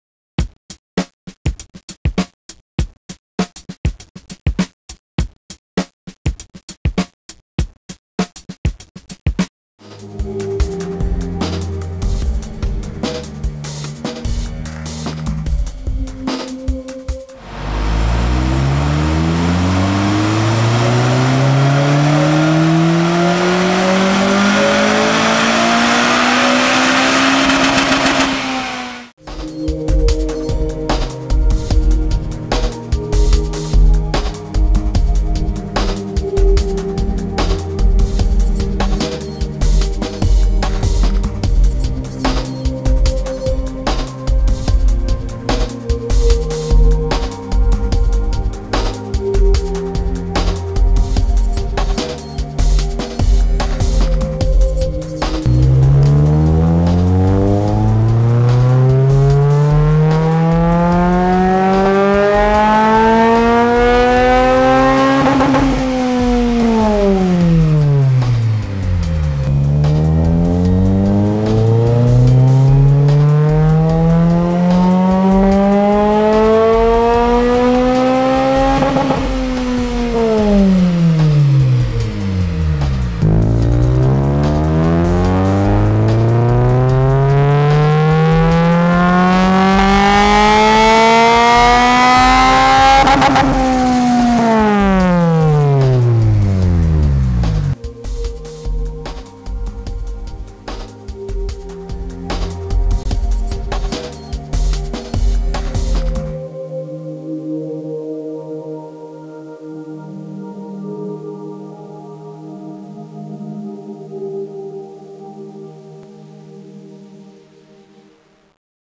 Skunk2 Mega Power R Exhaust for the 07 Honda Civic Si Sedan
Skunk2 is proud to introduce the Mega Power R Exhaust for the 2007+ Honda Civic Si Sedan featuring 70mm piping.